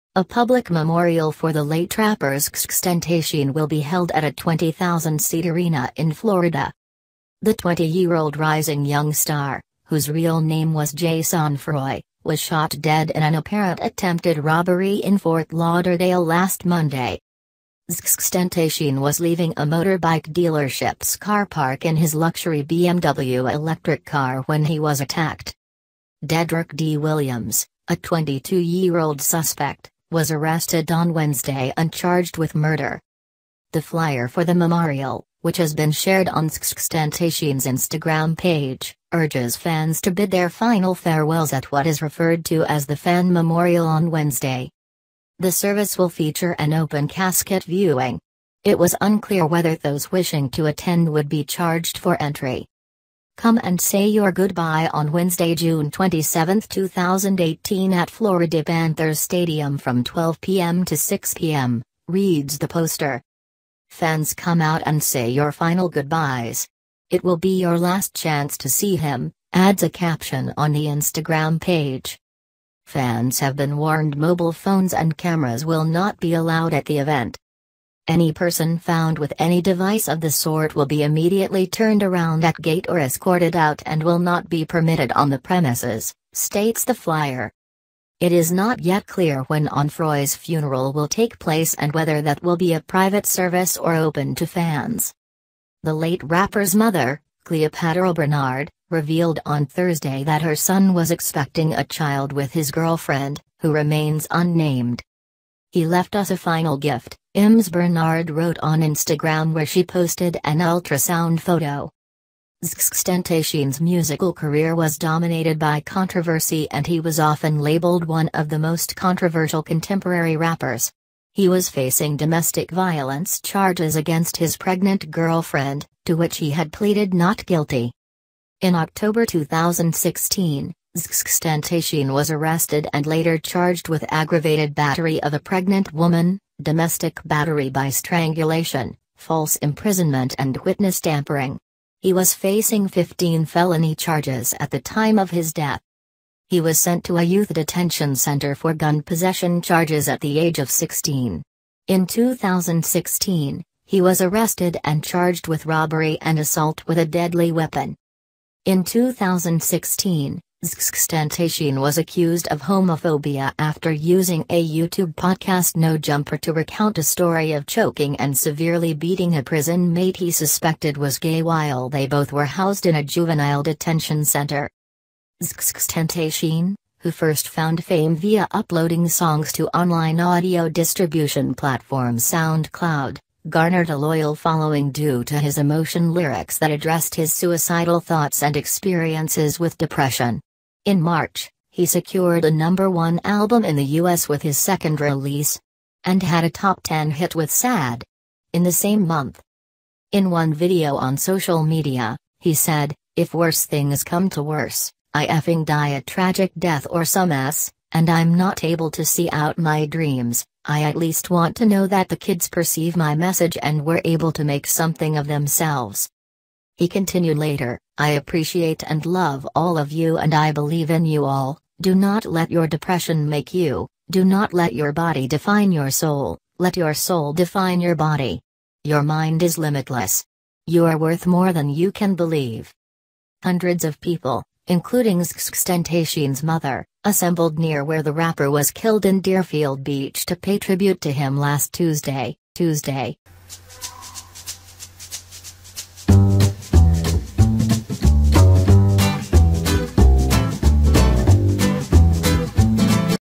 XXXTentacion murder: Memorial for rapper shot dead to be held at 20,000-seat arena in Florida
A public memorial for the late rapper XXXTentacion will be held at a 20,000 seat arena in Florida.

The 20-year-old rising young star - whose real name was Jaseh Onfroy - was shot dead in an apparent attempted robbery in Fort Lauderdale last Monday.

XXXTentacion was leaving a motorbike dealership’s car park in his luxury BMW electric car when he was attacked.

Dedrick D Williams, a 22-year-old suspect, was arrested on Wednesday and charged with murder.

The flyer for the memorial, which has been shared on XXXTentacion’s Instagram page, urges fans to bid their final farewells at what is referred to as the "fan memorial" on Wednesday.

The service will feature an open-casket viewing. It was unclear whether those wishing to attend would be charged for entry.

“Come and say your goodbye on Wednesday June 27th 2018 at Florida Panther’s Stadium from 12pm to 6pm,” reads the poster.

“Fans come out and say your final goodbyes. It will be your last chance to see him,” adds a caption on the Instagram page.

Fans have been warned mobile phones and cameras will not be allowed at the event.

"Any person found with any device of the sort will be immediately turned around at gate or escorted out and will not be permitted on the premises," states the flyer.

It is not yet clear when Onfroy's funeral will take place and whether that will be a private service or open to fans.

The late rapper’s mother, Cleopatra Bernard, revealed on Thursday that her son was expecting a child with his girlfriend, who remains unnamed.
celebrity news update,
Facebookpage:murreeview